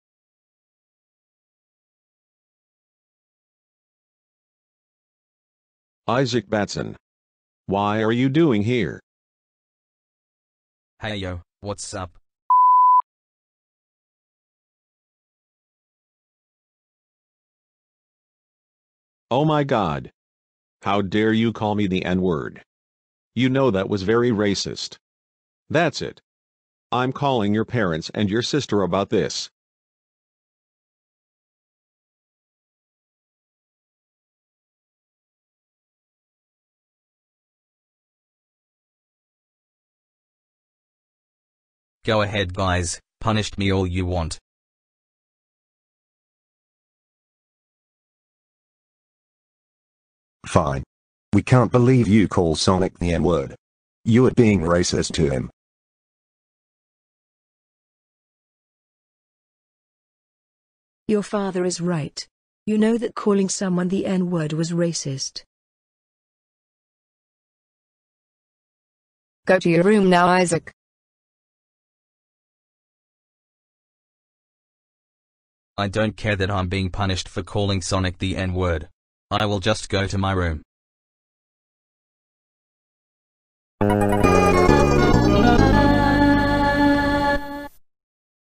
Isaac Batson Calls Sonic The Werehog The N Word
Note: That's Not A Grounded Video
Inspired By Vegeta The Princess Of All
Lesson: Don't Say The N Word